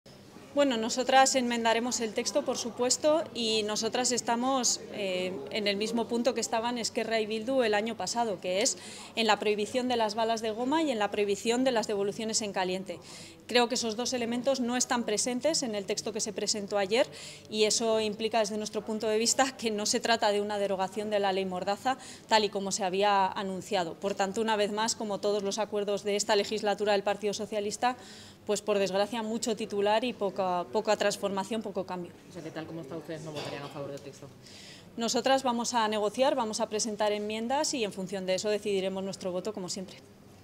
Podemos avisa al Gobierno de que no tiene su apoyo a la reforma de la Ley Mordaza
La secretaria general de Podemos, Ione Belarra, ha asegurado que el acuerdo entre Bildu y el Gobierno para reformar la Ley de Seguridad Ciudadana no supone una "derogación" de la denominada Ley 'Mordaza' y que, como todos los pactos impulsados por el PSOE en esta legislatura, supone "mucho titular y poca transformación".

(Fuente: Podemos)